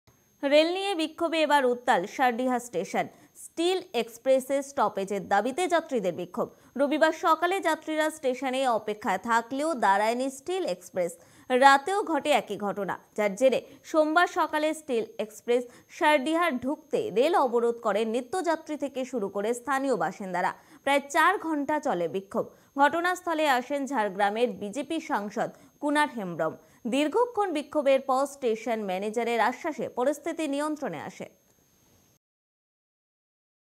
স্টপেজের দাবিতে বিক্ষোভ
রেল নিয়ে বিক্ষোভে এবার উত্তাল সারডিহা স্টেশন। স্টিল এক্সপ্রেসের স্টপেজের দাবিতে যাত্রীদের বিক্ষোভ। রবিবার সকালে যাত্রীরা স্টেশনে অপেক্ষায় থাকলেও দাঁড়ায়নি স্টিল এক্সপ্রেস। রাতেও ঘটে একই ঘটনা। যার জেরে সোমবার সকালে স্টিল এক্সপ্রেস সারডিহায় ঢুকতেই রেল অবরোধ করেন নিত্যযাত্রী থেকে শুরু করে স্থানীয় বাসিন্দারা। প্রায় ৪ ঘণ্টা চলে বিক্ষোভ। ঘটনাস্থলে আসেন ঝাড়গ্রামের বিজেপি সাংসদ কুনার হেমব্রম। দীর্ঘক্ষণ বিক্ষোভে পর স্টেশন ম্যানেজারের আশ্বাসে পরিস্থিতি নিয়ন্ত্রনে আসে।